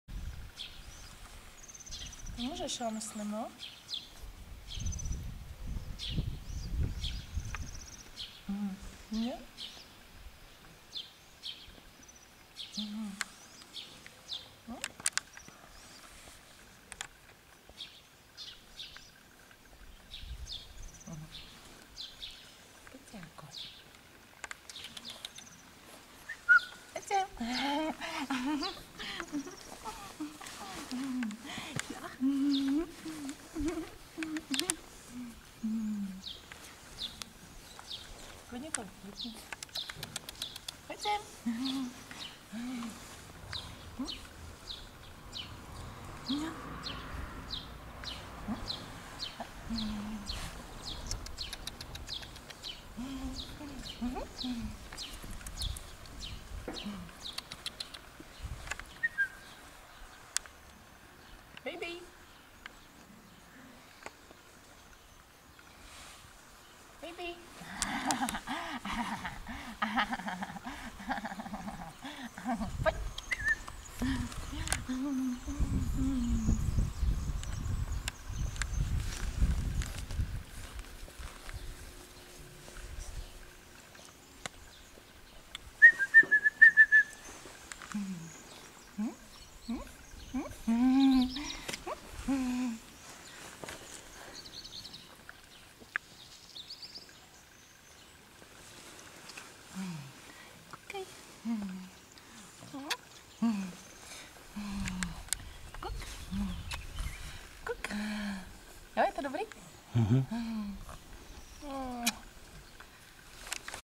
BabyBoy Diplomate Ambassadeur 2.7.2011
Baby Boy is 8 weeks old puppy (gold male). Baby Boy and his brindle brother Bandit Garepa were outside in the garden for the first time in their life :-) They enjoyed it!